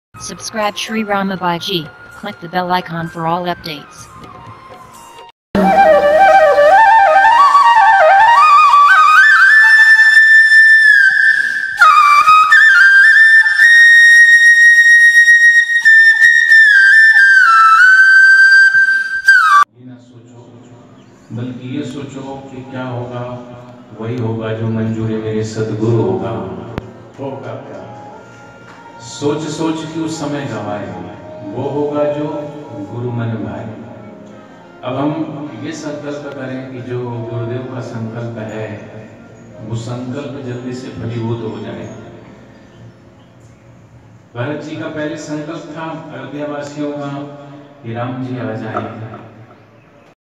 31 May 2017- Satsang- Sant Shri Asharam Ji Bapu ashram Dhulia ,Maharashtra
परम पूज्य संत श्री आशाराम जी बापू के साधक शिष्य श्री रामा भाई जी के सानिध्ये मे भव्य सत्संग - 🕉

🗓 दिनांक - 31 मई 2017

📍स्थान - संत श्री आशाराम जी बापू आश्रम ,धुलिया ,महाराष्ट्र ।